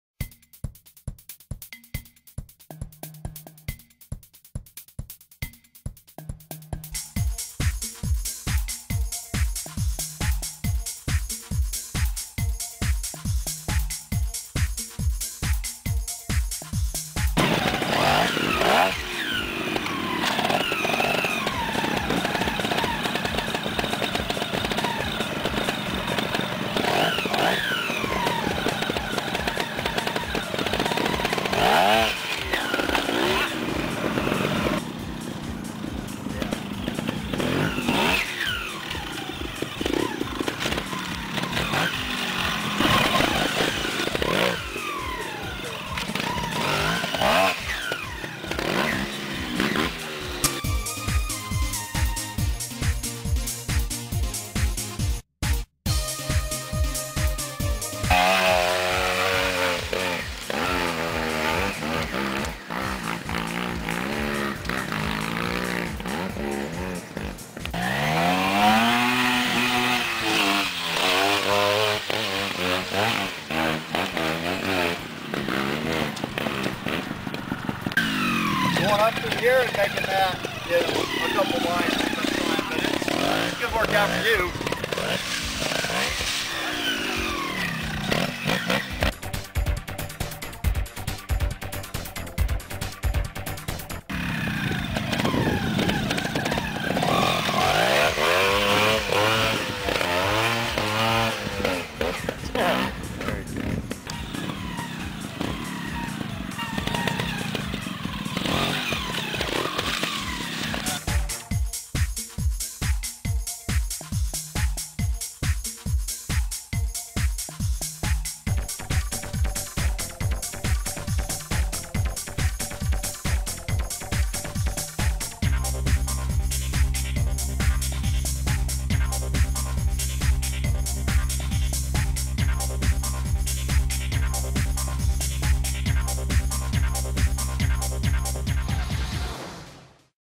FIRST USA TESTING - APRIL 16 2016 - TILTON ILLINOIS, TRS One TRIALS with Jesse Wellenstein
Saturday the 16th of April 2016 - We test the new TRS One (300cc) in the forest.